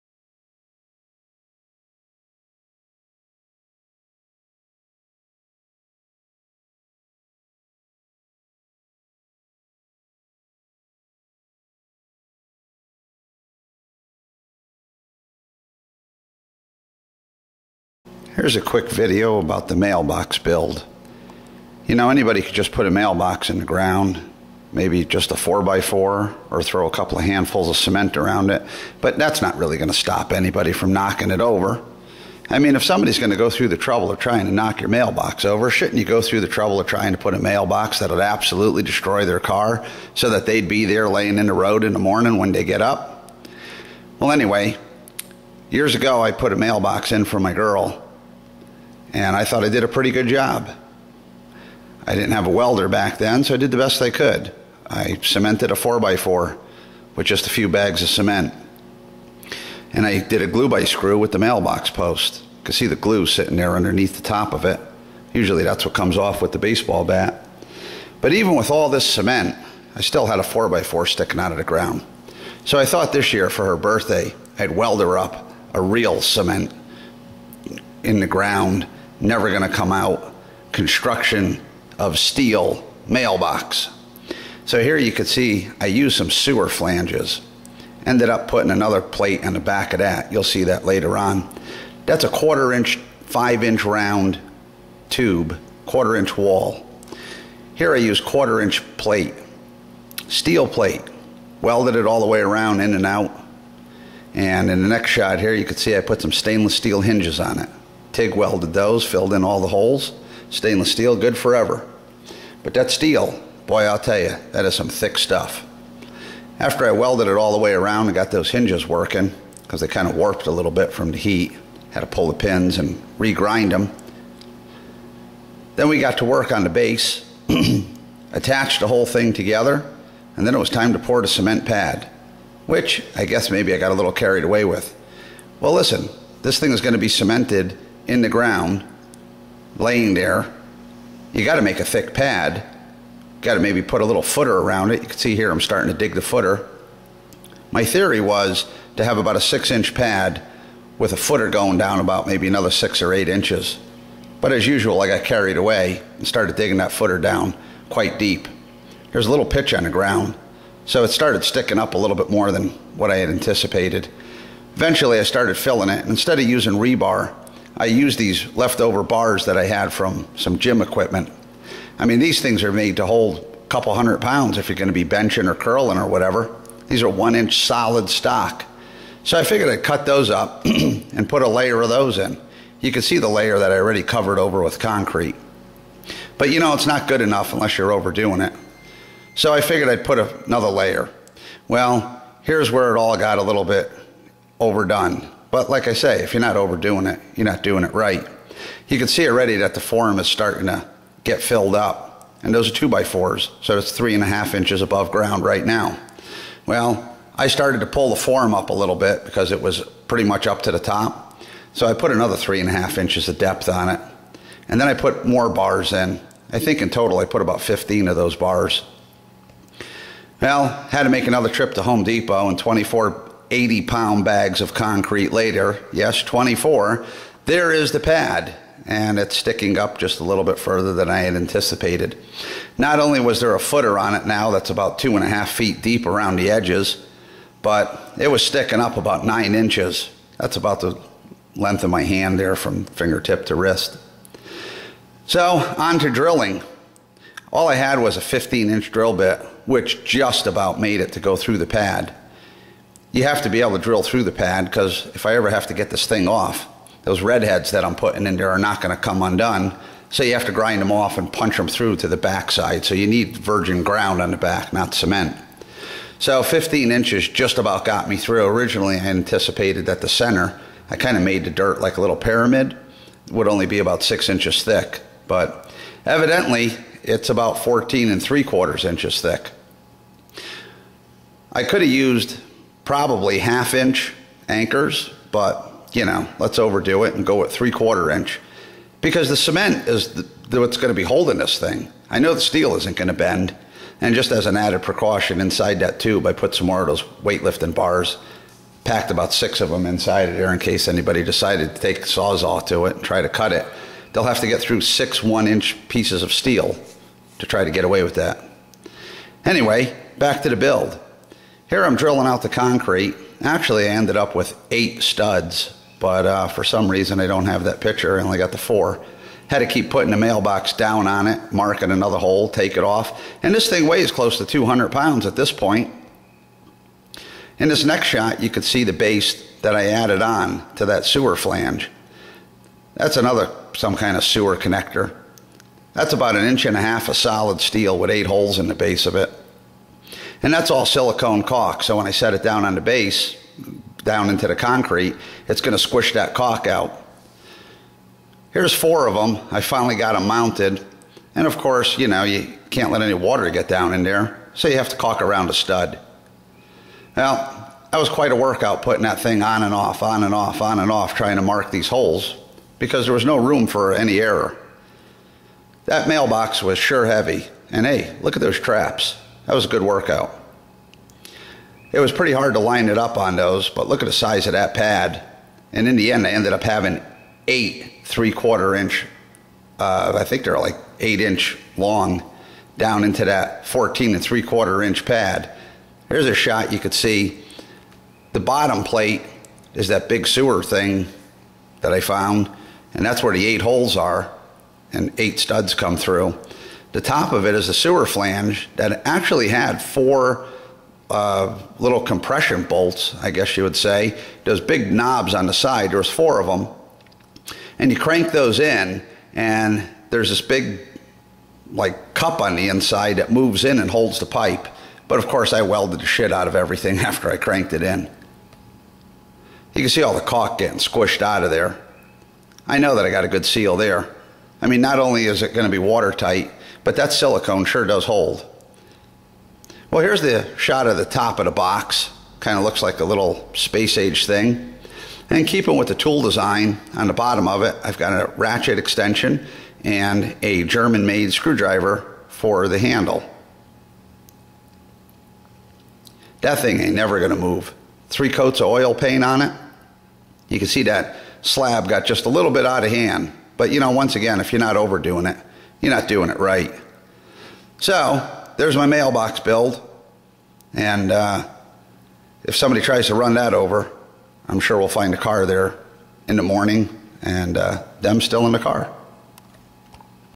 the mailbox build, for my guys out of state, yes it's ALL STEEL
don't know how else to say it,  it's all steel and yes it really took 24
80lb bags of concrete,
 why do you guys keep asking about a 4x4?
cause if you ain't over doin' it,   you ain't doing it right